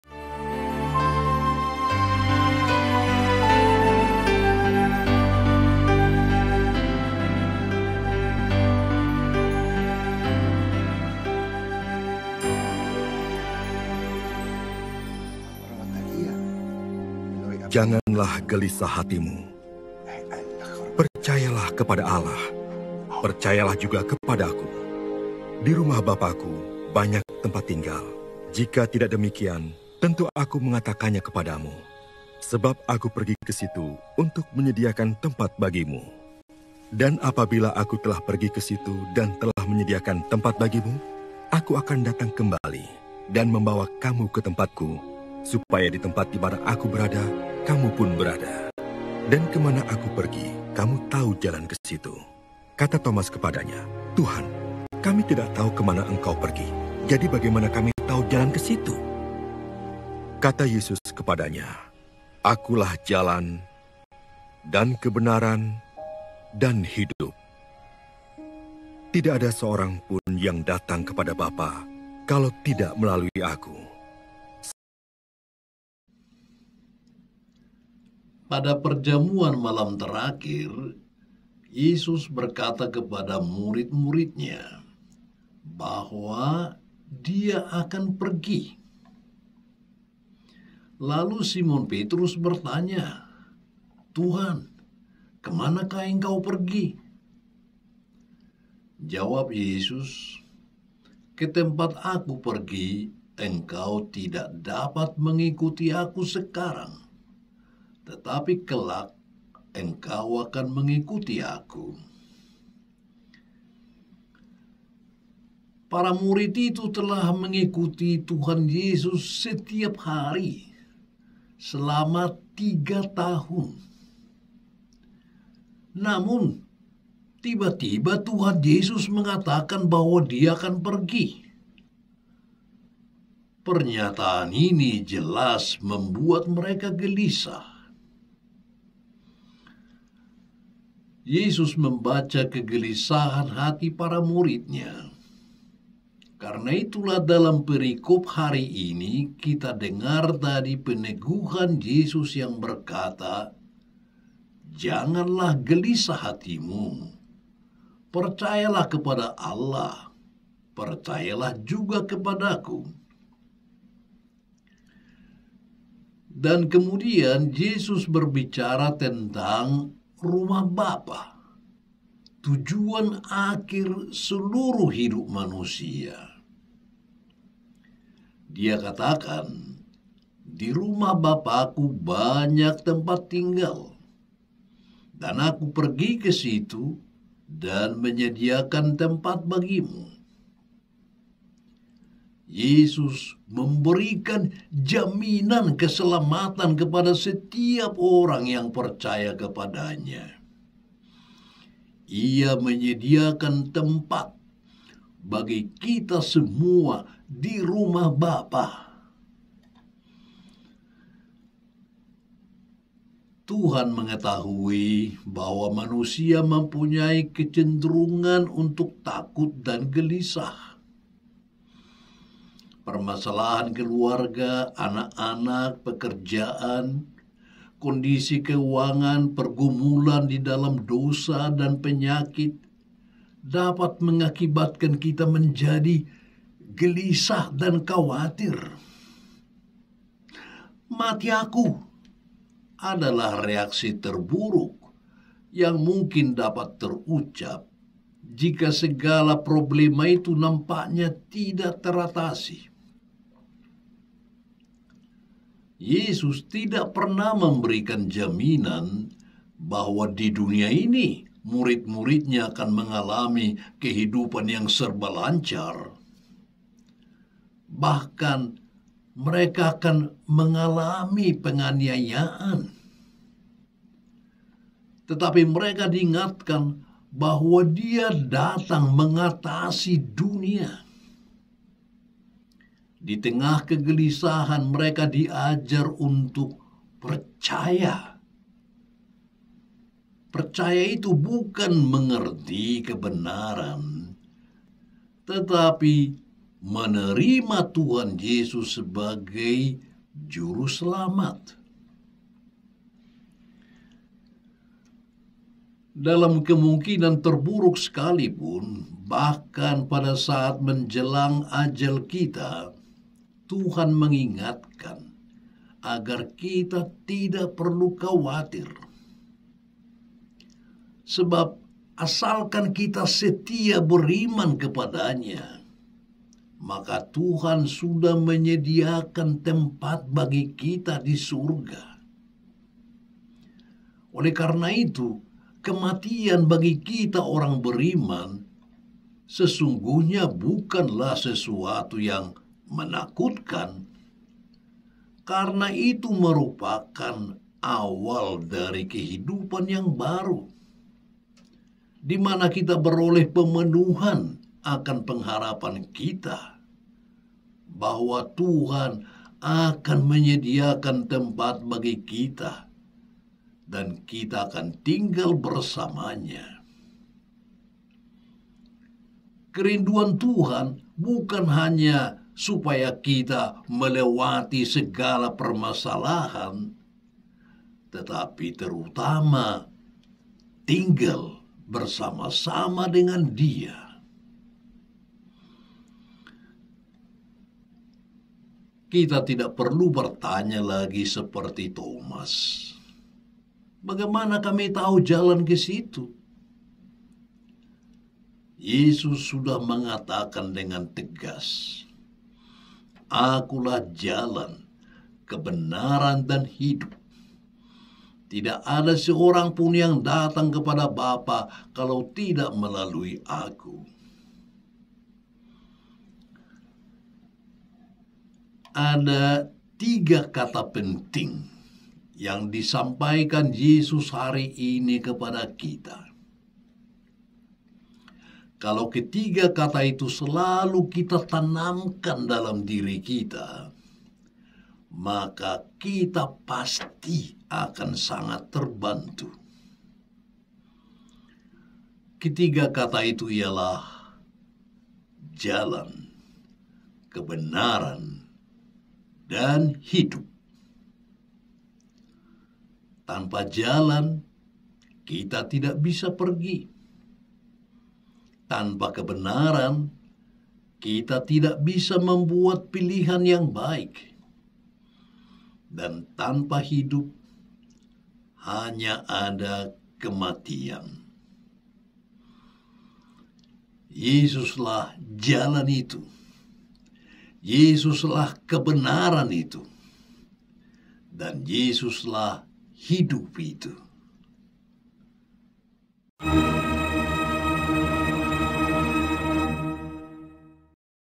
AKULAH JALAN, KEBENARAN DAN HIDUP. Santapan Rohani Jumat Paskah 4
RP Leo Sipahutar OFMCap
Ada tiga kata penting yang disampaikan Yesus hari ini kepada kita. Kalau ketiga kata itu selalu kita tanamkan dalam diri kita, maka kita pasti akan sangat terbantu. Ketiga kata itu ialah: jalan, kebenaran dan hidup.

Tanpa jalan kita tidak bisa pergi, tanpa kebenaran, kita tidak bisa membuat pilihan yang baik dan tanpa hidup, hanya ada kematian.
Yesuslah jalan itu, Yesuslah kebenaran itu dan Yesuslah hidup itu.